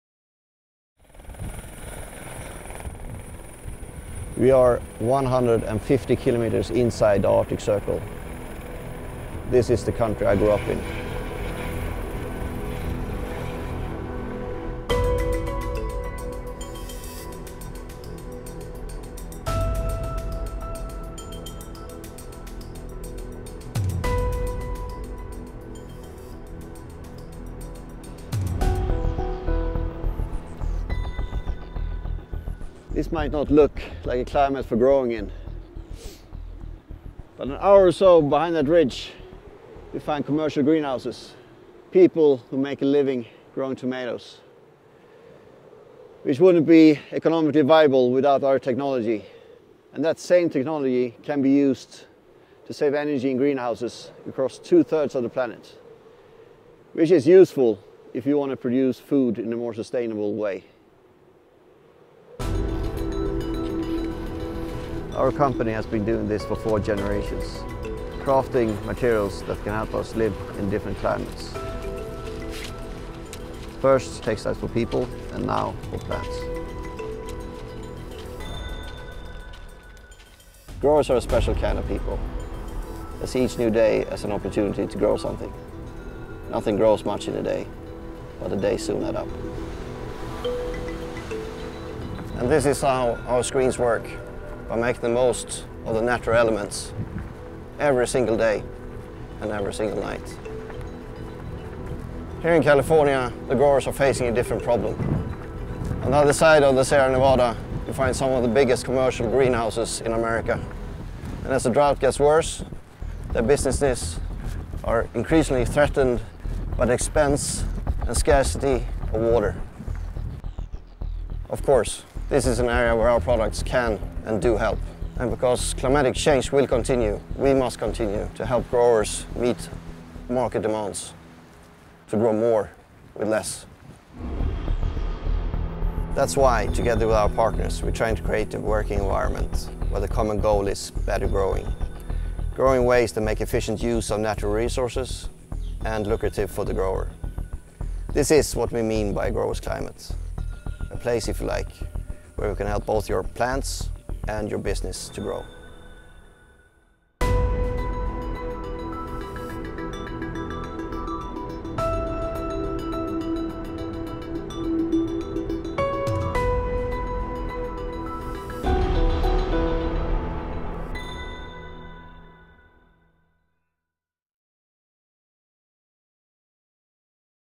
Svensson - Walking in a Grower's Climate
A Swedish businessman walks a glacier and a lakebed to speak to the greenhouse growing industry and the food market. Can better growing help the planet?

In May 2014 Anders Ludvigson - Executive Vice President of Ludvig Svensson - took a small film team to Sweden's highest mountain while it was still in the grip of winter. A week later he continued his micro-adventure on the edge of California's Mojave desert - following the worst drought in fifty years. While both environments appear extreme, neither are far from vegetable production sites. Svensson climate screens are used in both regions to reduce energy and water consumption - doesn't the rest of the world need to conserve resources and grow food more efficiently?

Credits;
Director: Simon Coe
Photographers: Byron Shah, Ola Westberg, Jon Hope
Drone Pilot: Donovan Davis
Editing: Ola Westberg
Production Assistant: Jordan Loraine
Graphic Designer: Peter Wettéus

Viktoria Lundqvist and STF Kebnekaise
Kallax Flyg